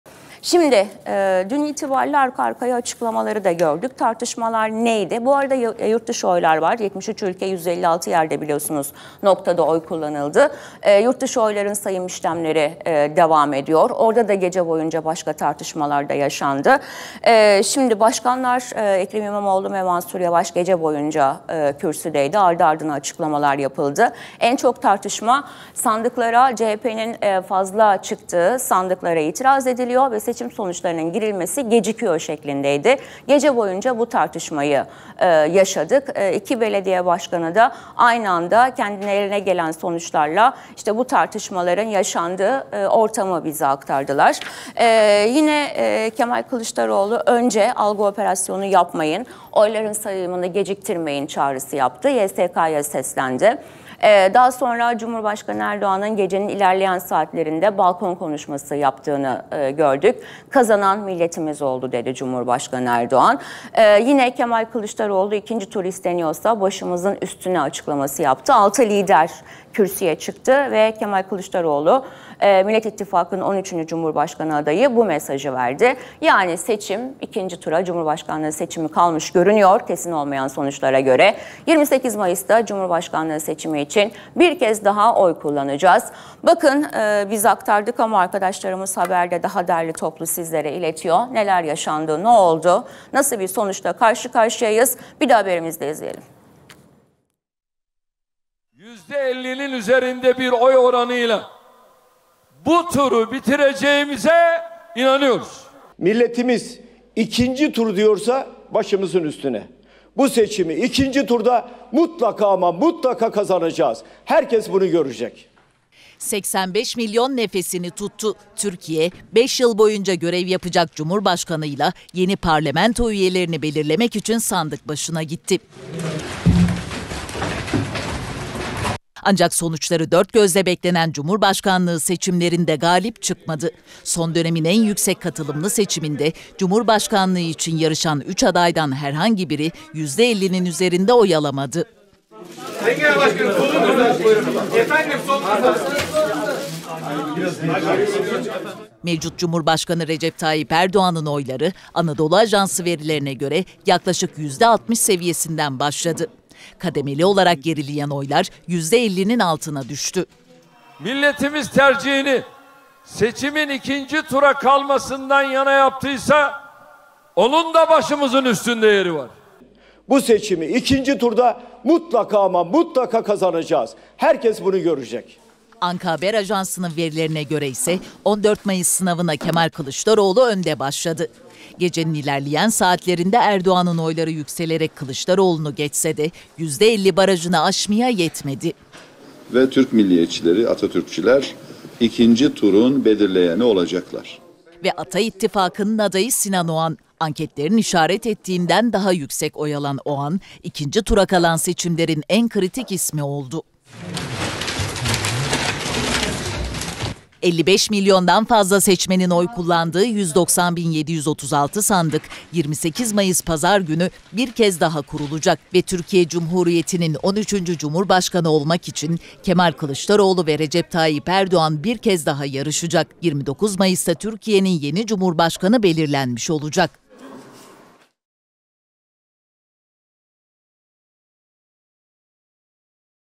YSK Oyların Sayımını Geciktirdi mi? Kemal Kılıçdaroğlu'ndan Çarpıcı İkinci Tur Mesajı
Digiturk: 60
D-smart: 93
Turkcell TV Plus: 69
Tivibu: 69
Türksat Kablo TV: 76